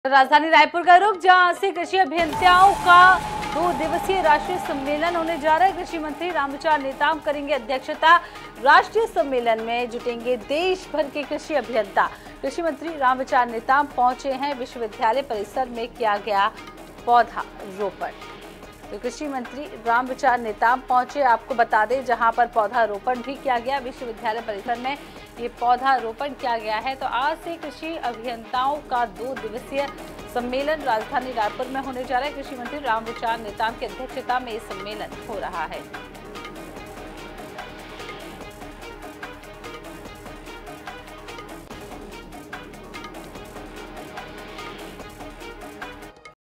आज से कृषि अभियंताओं का दो दिवसीय राष्ट्रीय सम्मेलन | कृषि मंत्री Ramvichar Netam करेंगे अध्यक्षता
INH, आज से कृषि अभियंताओं का दो दिवसीय राष्ट्रीय सम्मेलन | कृषि मंत्री Ramvichar Netam करेंगे अध्यक्षता